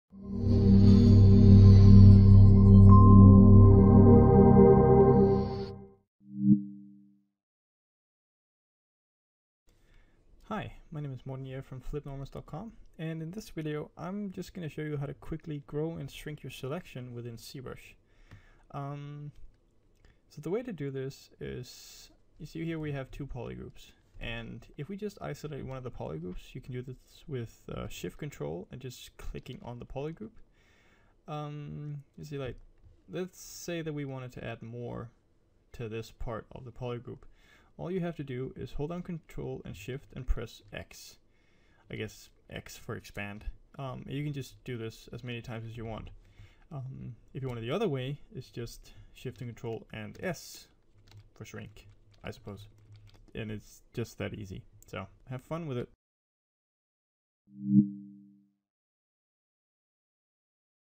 Zbrush Sculpting Tips and Tricks Tutorial - Shrink and Grow Selection HD
In this tutorial we'll take a look at how you can quickly shrink and grow your selection in ZBrush.

This Zbrush Tutorial was created by the guys from FlippedNormals.com so If you enjoyed this tutorial make sure you  check them out here: